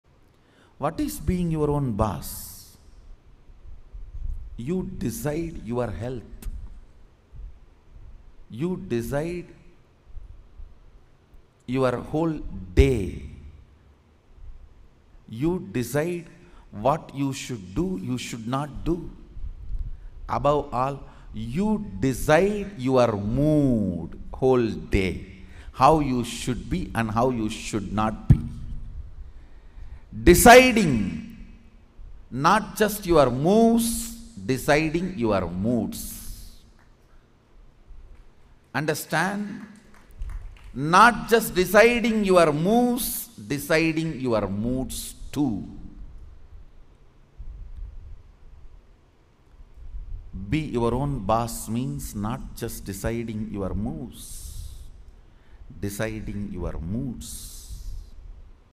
What is being your own Boss? Deciding not just your moves but your moods too
What is being your own Boss? Deciding not just your moves but your moods too His Divine Holiness Bhagwaan Sri

Original Video
Be Your Own Boss Develop the Right Cognition